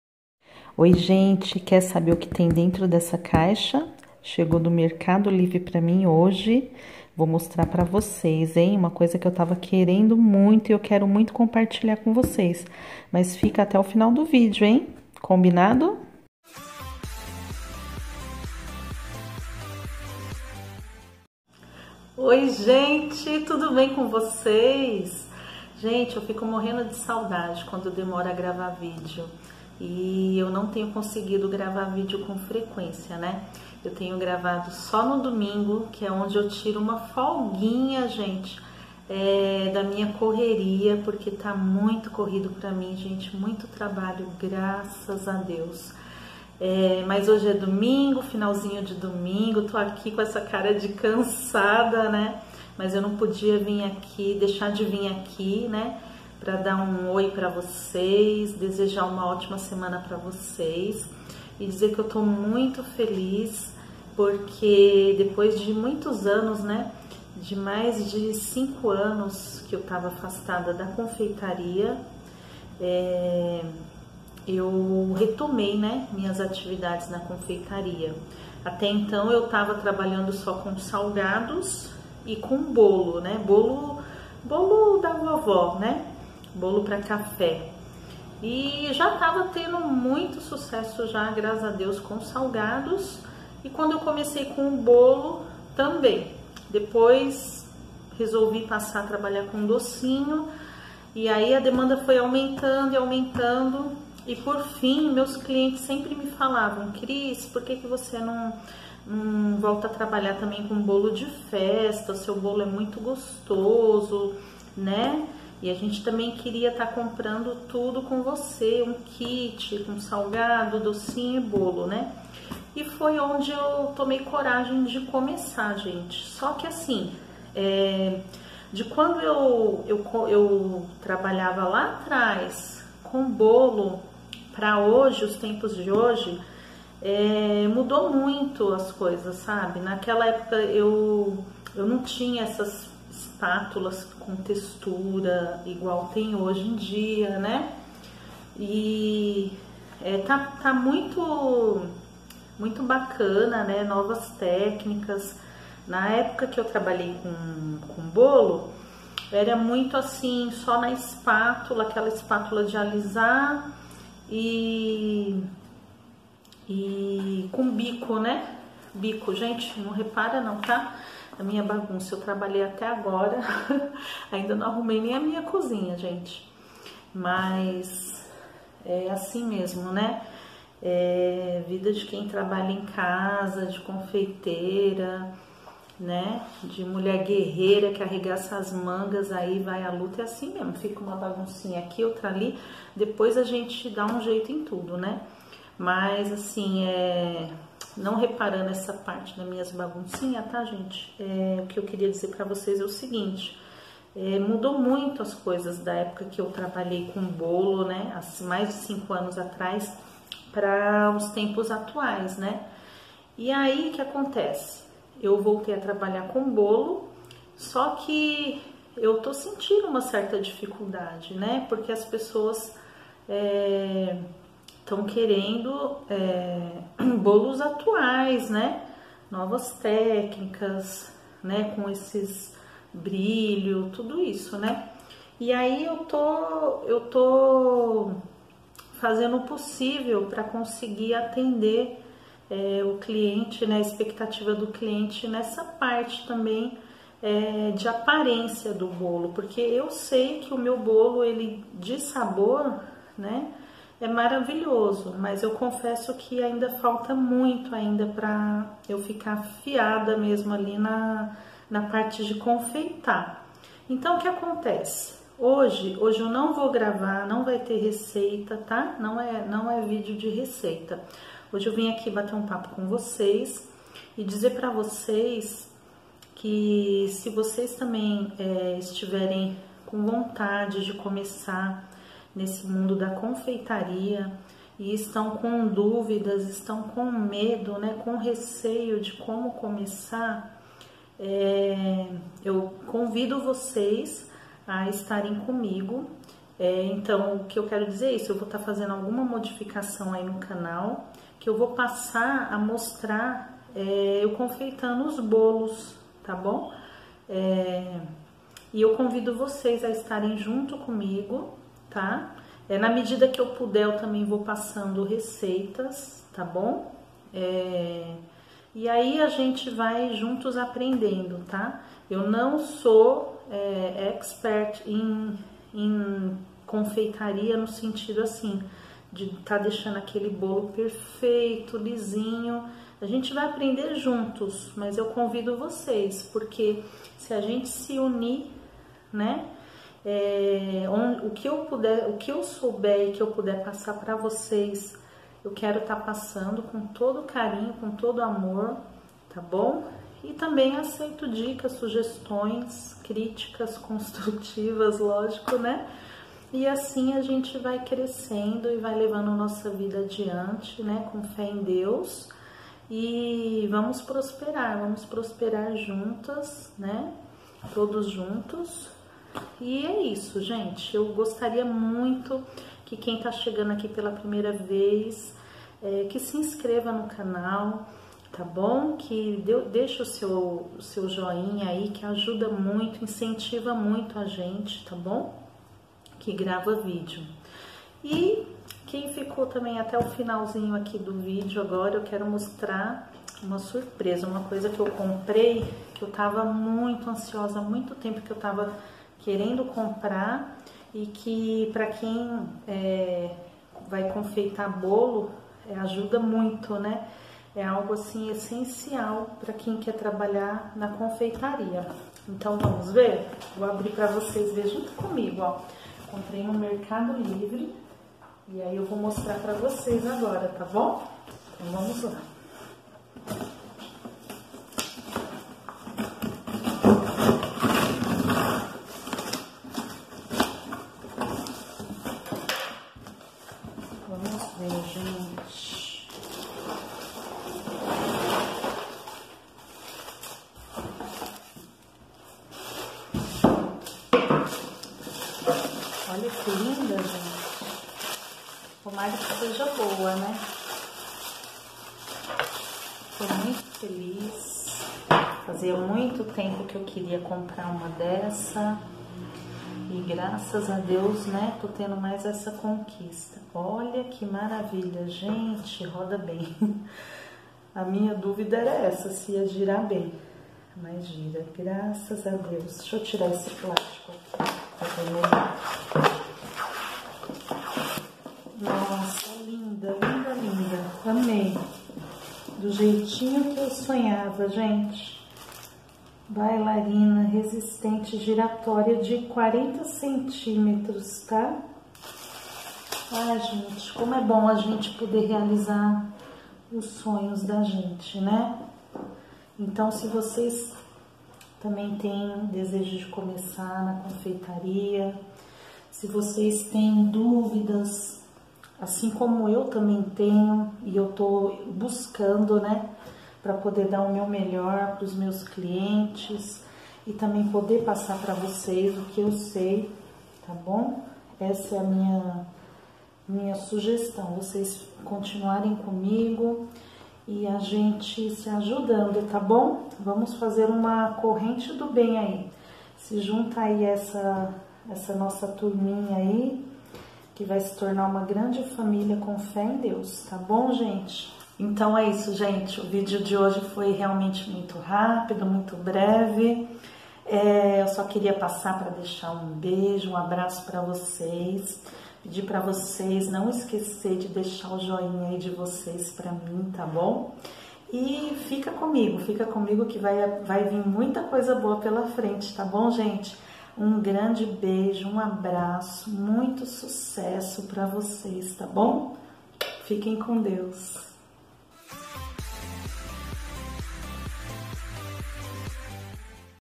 FINALMENTE CHEGOU!!
Hoje vim aqui bater um papo com vocês, contar as novidades, e mostrar uma surpresa que chegou pra mim hoje e que alegrou muito meu dia!!! 😍

E fico muito feliz em poder compartilhar vom vocês todas as minhas conquistas, por menor que sejam tá bom!

Um abraço e sucesso pra nós!!!

Ah gente, me segue lá no insta onde posto todos os meus trabalhos tá bom!